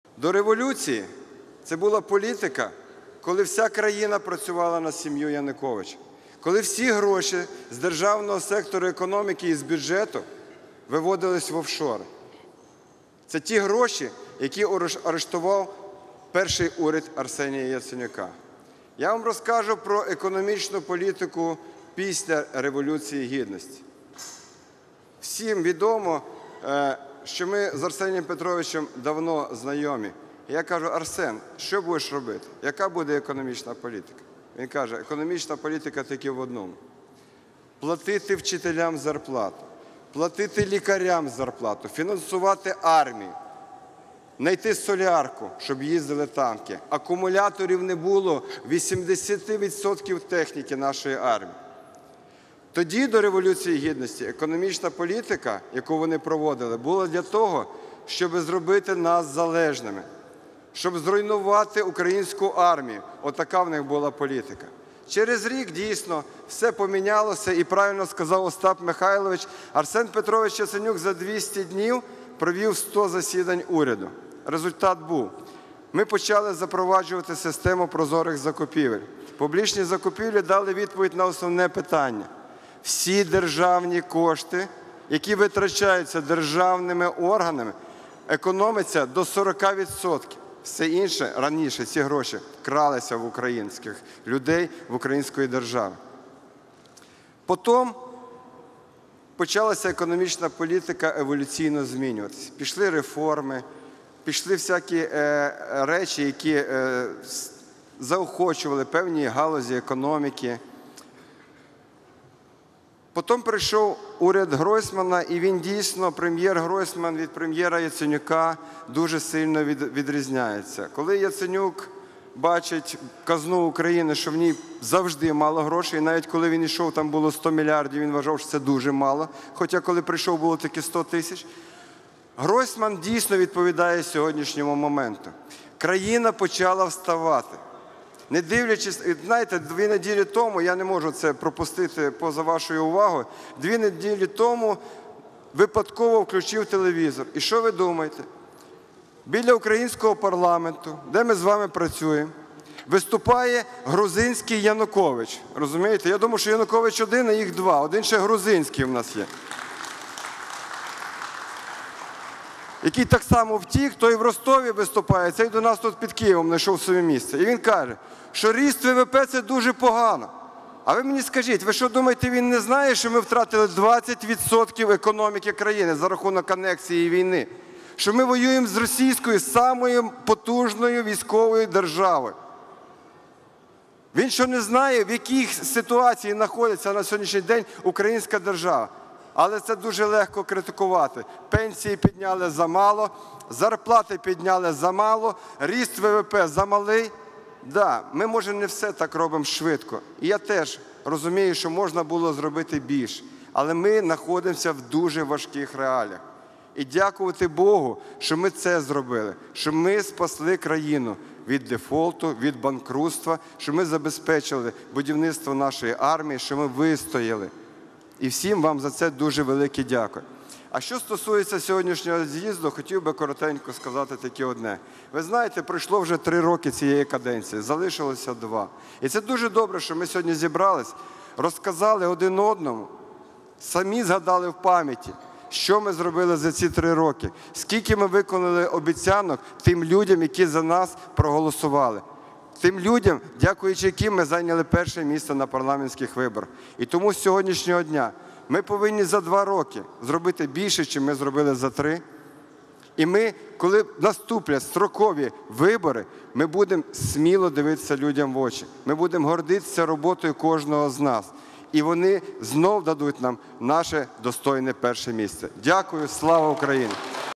Економічна політика країни має полягати в добробуті кожного с, - А.Іванчук
Економічна політика України полягає у тому, щоб економіка працювала на розвиток держави і її громадян.

На цьому наголосив заступник голови фракції "Народний фронт", Голова комітету з питань економічної політики Андрій Іванчук під час  з’їзду «Народного фронту» в суботу, 11 листопада.

"До Революції гідності - це була політика, коли вся країна працювала на сім’ю Януковича. Коли всі гроші з державного сектору, економіки і бюджету виводилися в офшори. Економічна політика Уряду Арсенія Яценюка полягала у виплаті зарплат вчителям, лікарям, фінансуванні армії", - сказав народний депутат.

За його словами, політика, яку проводили до Революції гідності, полягала у тому, щоб зробити країну залежною і зруйнувати її обороноздатність, але "Уряду Яценюка вдалося відвести ці загрози і розпочати проводити  реформи".

"Ми запровадили систему прозорих закупівель і це дозволяє економити до 40% державних коштів. Економіка почала еволюціонувати і це дало поштовх для багатьох галузей. А раніше ці кошти просто розкрадалися", - зазначив Голова економічного комітету.

Андрій Іванчук зауважив, що уряд Яценюка і уряд Гройсмана знаходились в різних умовах і перед ними були різні виклики. На момент призначення уряду Яценюка в державній казні було 100 тисяч гривень, а коли він йшов із посади – майже 100 мільярдів. І це дало змогу нинішньому уряду проводити реформи в країні.

"Окремі політикани заявляють, що ріст ВВП поганий. Ви думаєте вони не знають, що ми втратили 20% економіки країни за рахунок анексії і війни? Що ми воюємо з Росією, в якої одна із самих потужних армій в світі. Не розуміють, в якій ситуації знаходиться на сьогодні українська держава?", - обурився депутат.

"Дуже легко критикувати, пенсії підняли замало, зарплати - замало, ріст ВВП малий. Я розумію, що можна було зробити більше, але ми знаходимося в дуже важких реаліях. Варто подякувати, що нам вдалося врятувати державу від дефолту, що ми забезпечили потреби армії і змогли вистояти", - підкреслив він.

Завершуючи  виступ,  Іванчук сказав, що залишилось два роки роботи парламентської фракції. З’їзд - це гарна  нагода пригадати, що вже зроблено і що необхідно виконати за наступні два роки.

"Сьогодні ми згадали, що ми зробили за ці три роки. Скільки ми виконали обіцянок, які ми давали тим людям, котрі за нас голосували. Тим людям, завдяки яким ми зайняли перше місце на парламентських виборах 2014 року. Із сьогоднішнього дня, ми повинні за 2 роки зробити більше, ніж вже зроблено. І на наступних виборах, ми сміливо будемо дивитися людям в очі і гордитися роботою кожного з  нас. І вони знову дадуть нам наше достойне перше місце", - резюмував Андрій Іванчук.

Приєднуйся!